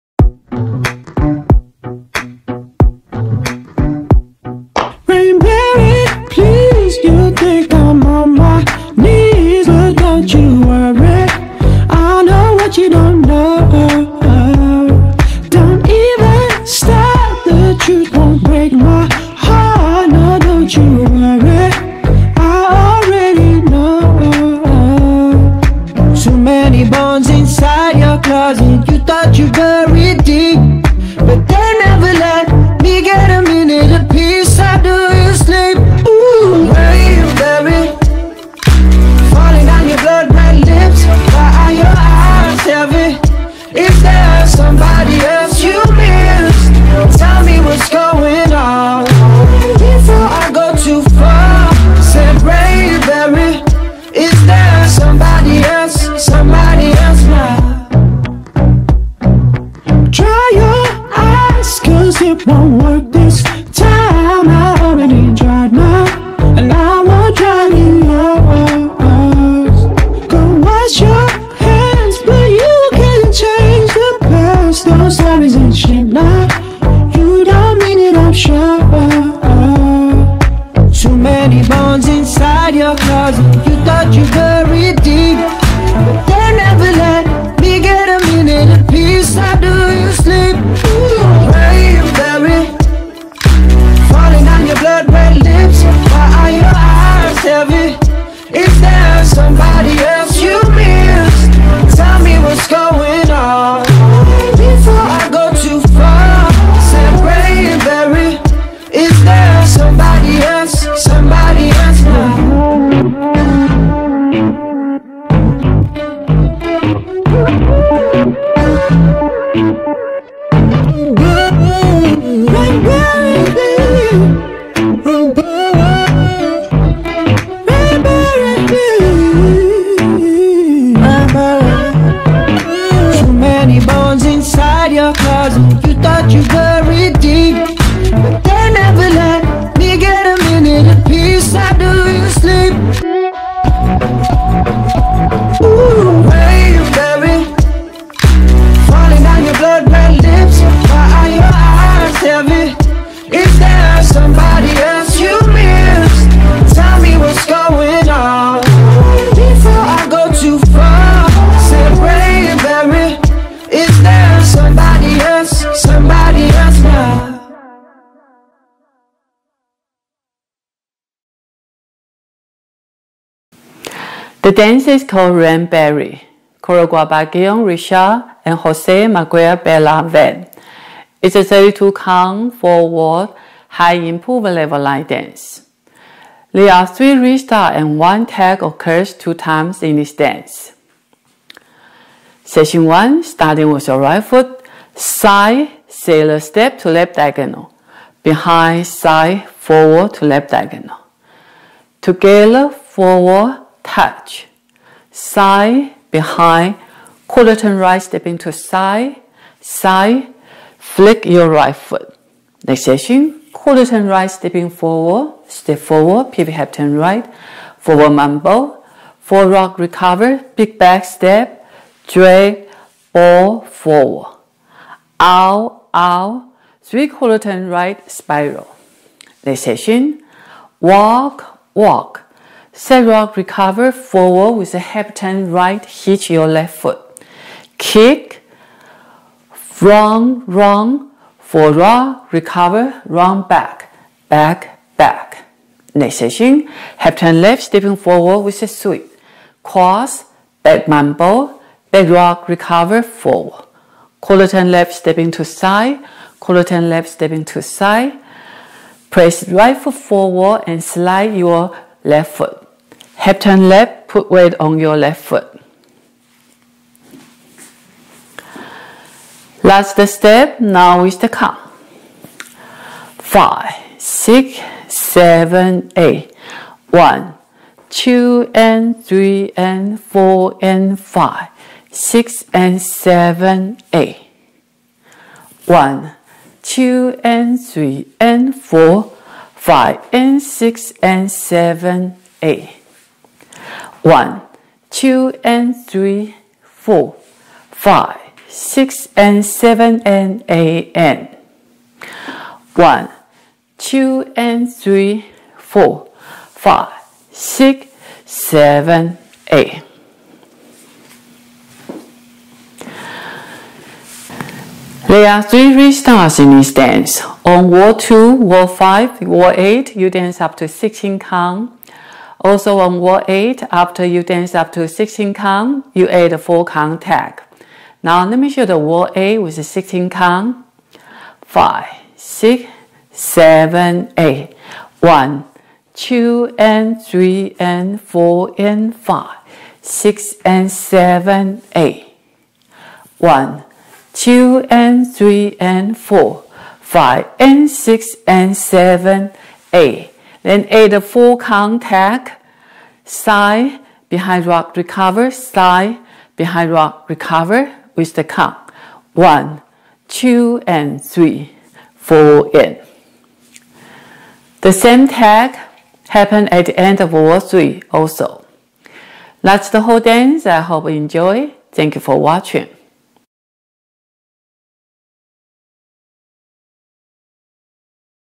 Rainberry - Line Dance (Dance & Teach in English)
rainberry line dance
Description : 32 Counts, 4 Walls, High Improver
Choreographer :
Guillaume Richard & Jose Miguel Belloque Vane